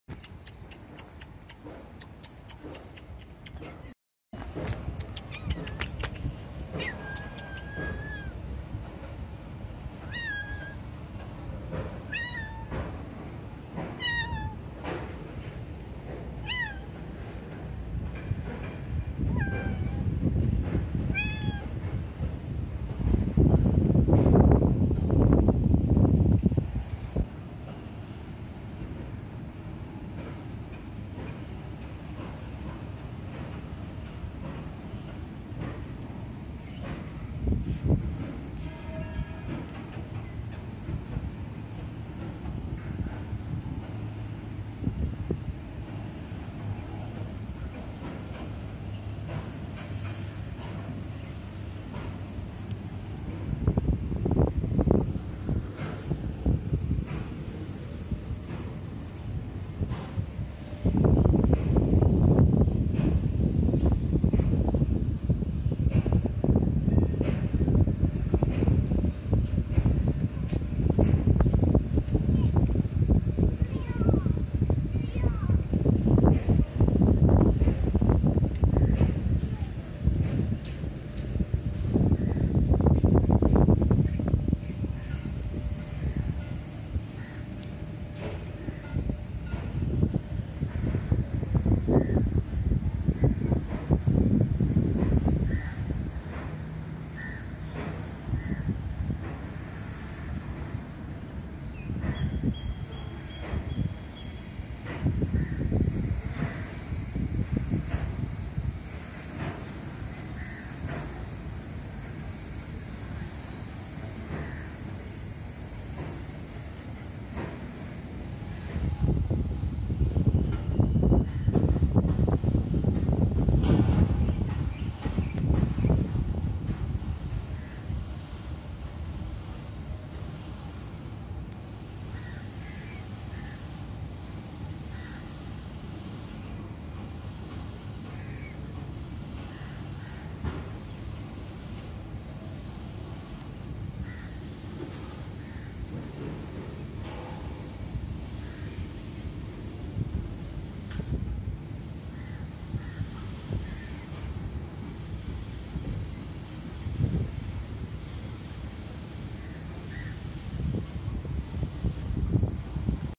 Cats Sometimes Just Want a Little of Your Attention, Not Always Food 😊
Shot with Asus Zenfone 5